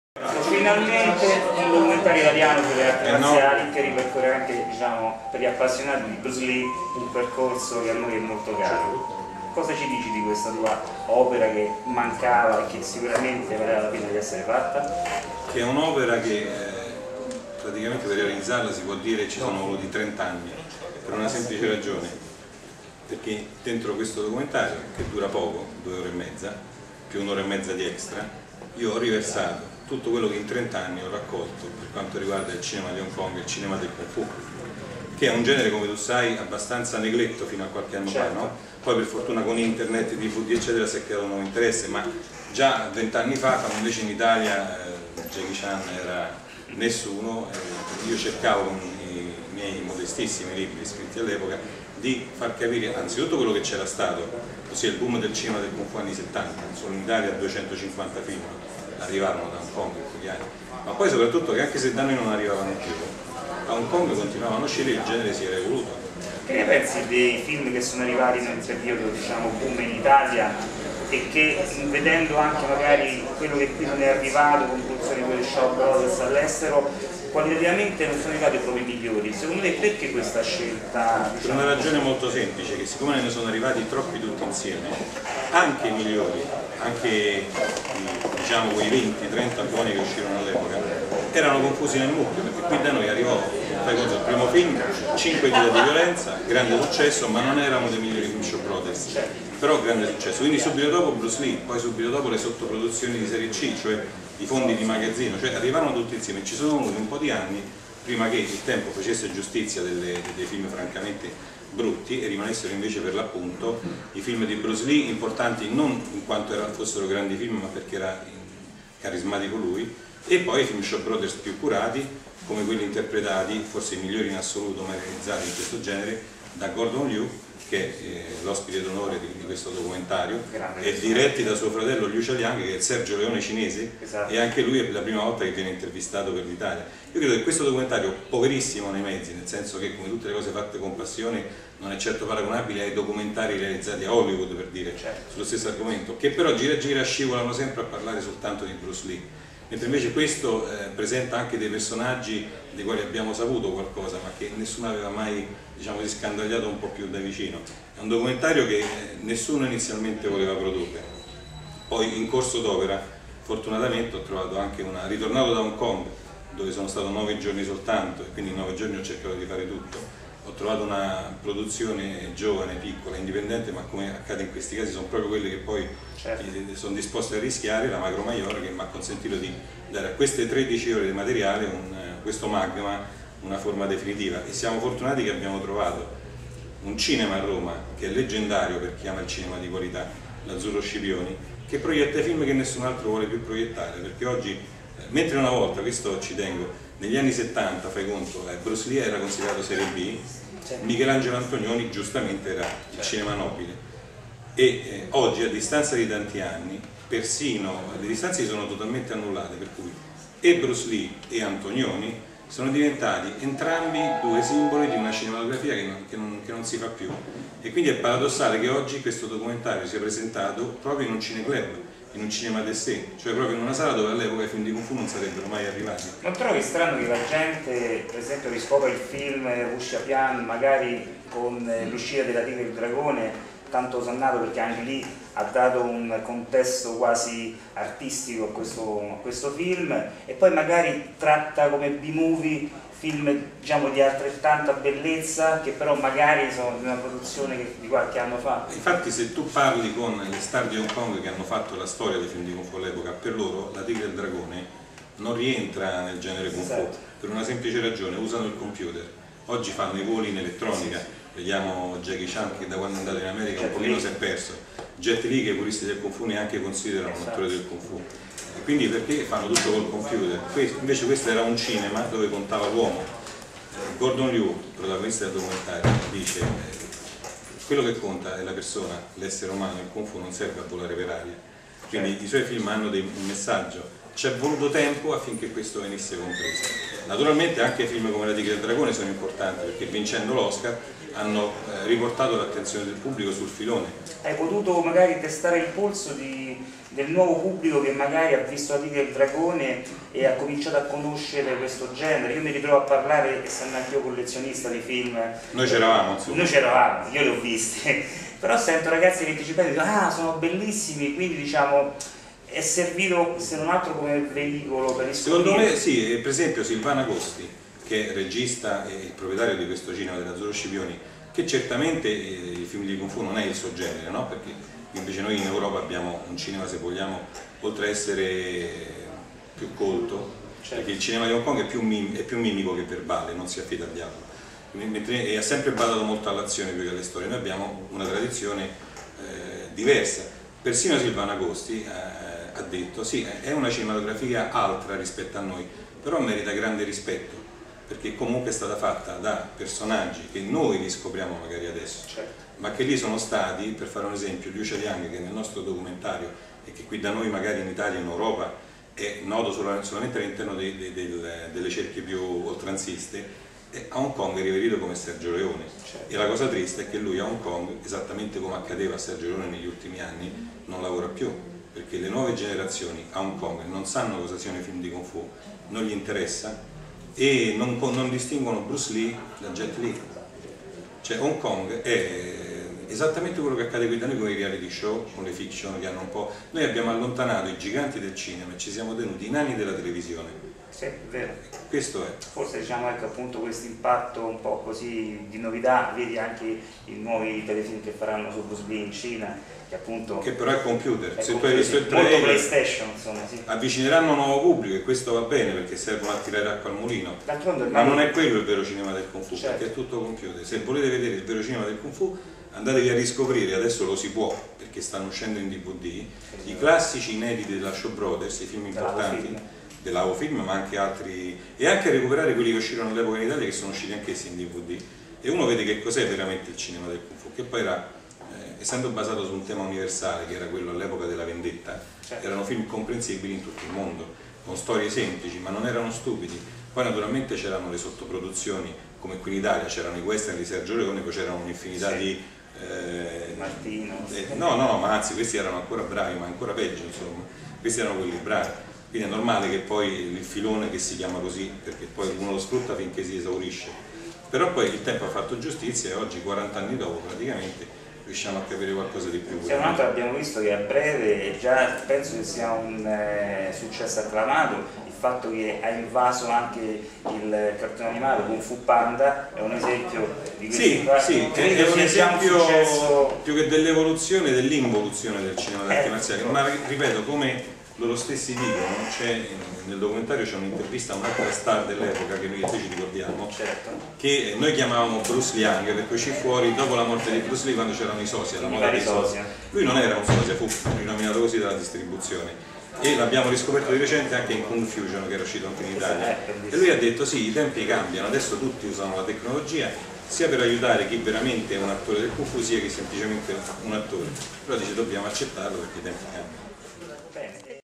Intervista all'autore Lorenzo de Luca sui Kung Fu Movie
Intervista per Proiezioni Mentali del master Roberto Castagnini all'autore e sceneggiatore Lorenzo de Luca i occasione della presentazione del documentario da lui scritto e diretto "L'Urlo di Chen Terrorizza ancora l'Occidente" al cinema Azzurro Scipioni.